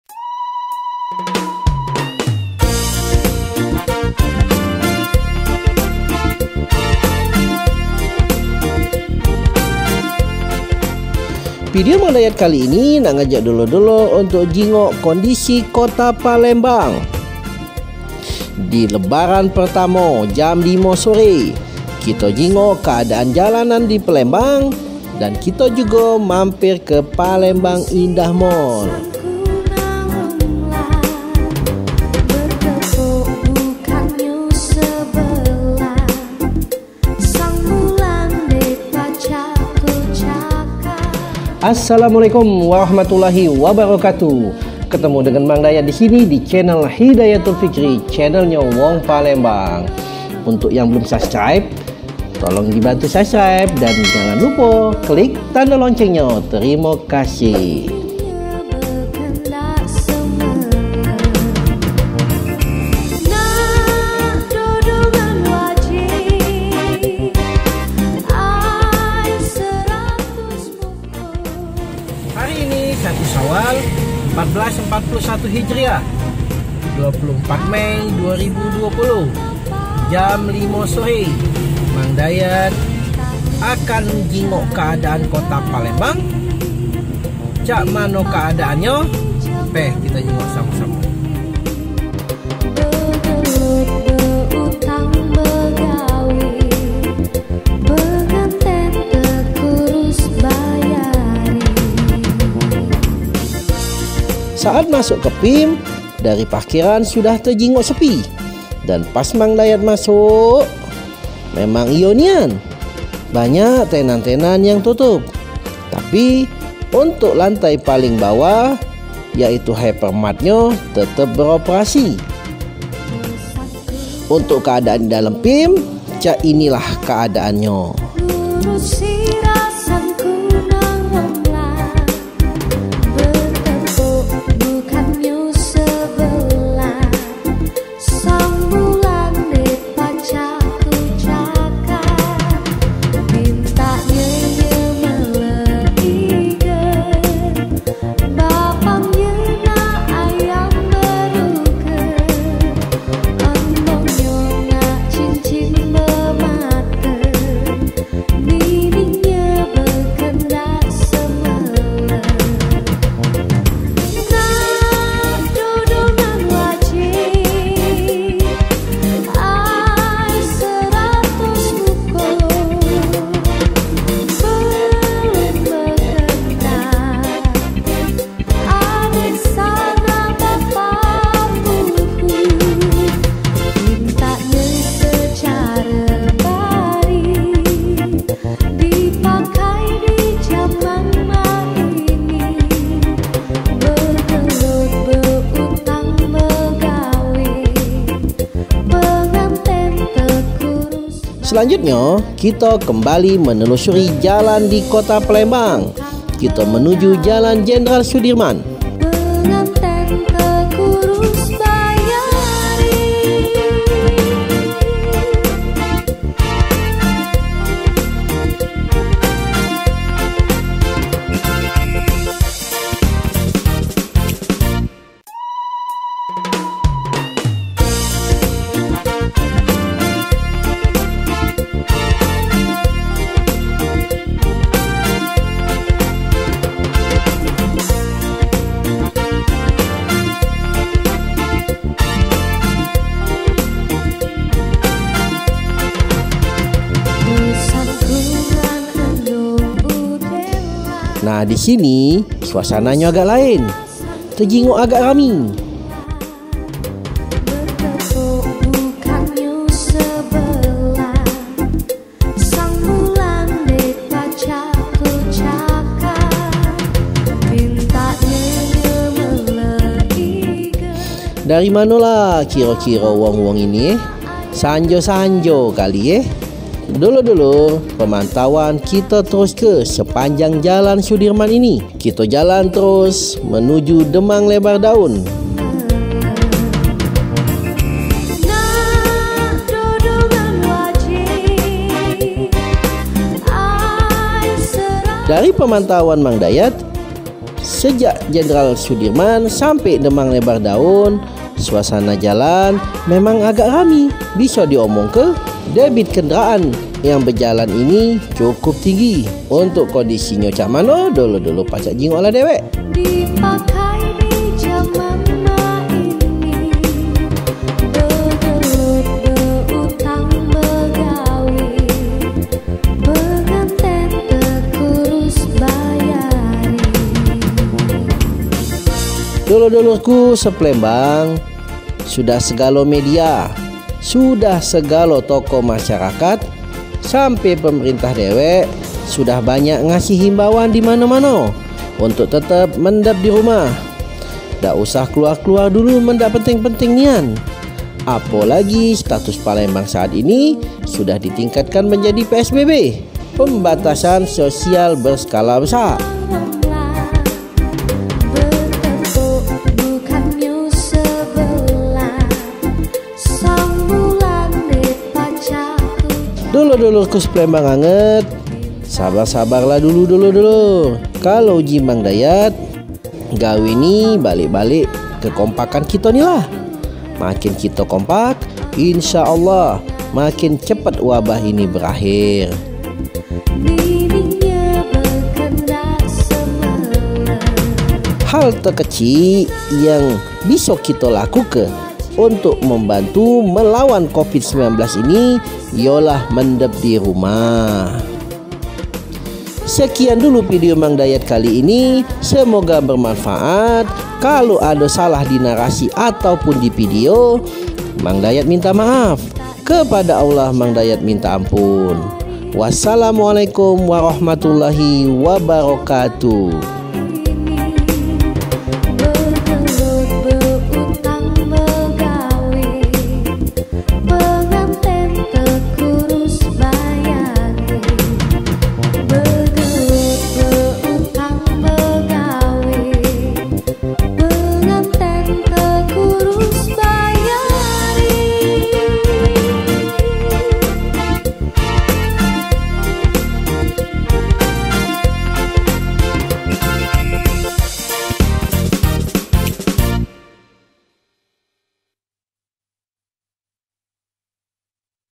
Kondisi Palembang di hari pertama Idul Fitri 1441H ditengah ditetapkannya PSBB mangdayat episode 30
Mang dayat Episode 30
Mang Dayat cerito episode 19
video Kali ini Mang Dayat mengajak subscriber untuk melihat kondisi Kota palembang di Hari Raya idul Fitri 1441 H hari pertama

dalam pemantauan mang Dayat Sore hari jam 5 beberapa daerah terlihat lenggang namun kondisi yang berbeda di jalan poros utama Kota Palembang yaitu Jalan Jendral sudirman yang terlihat ramai

selain memantau kondisi jalanan Mang Dayat juga mampir ke Palembang Indah Mall atau PIM. Untuk Mall terlihat sepi dan banyak tenant yang masih memilih tutup, namun untuk hypermart masih beropersi seperti biasa

mang Dayat juga mengajak Masyarakat Kota Palembang untuk mengikuti anjuran pemerintah untuk tetap dirumah agar bersama melawan wabah ini dimulai dari hal terkecil mengambil peranan membantu dengan tinggal dirumah

sekian dari Mang Dayat, selamat hari Raya Idul Fitri 1441 H, Mohon maaf lahir dan batin